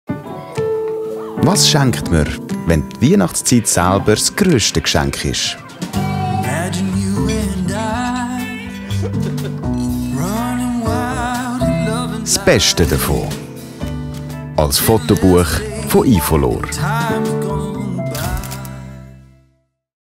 ifolor Inspiration | The best Christmas present. An ifolor photo book.
What do you give when Christmas time itself is the greatest gift?
The best of it in a photo book from ifolor.